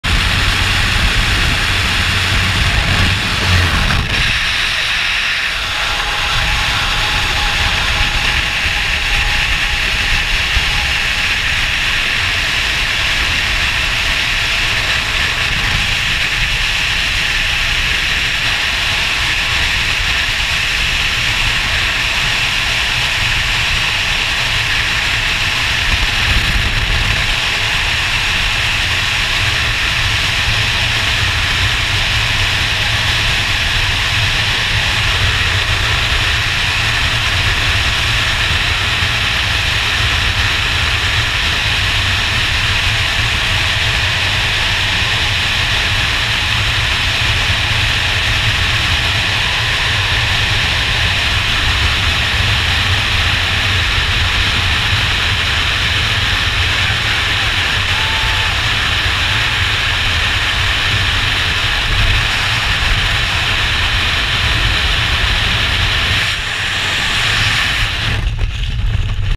EESTI REKORD: 28-liikmeline kujund vabalangemises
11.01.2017 püstitasid 29 Eesti langevarjurit Portugalis uue rahvusliku rekordi, moodustades Skydive Algarve kohal vabalangemises 28 sportlasest koosneva kujundi. Kaameramees, nagu ikka, lendas kujundi kohal.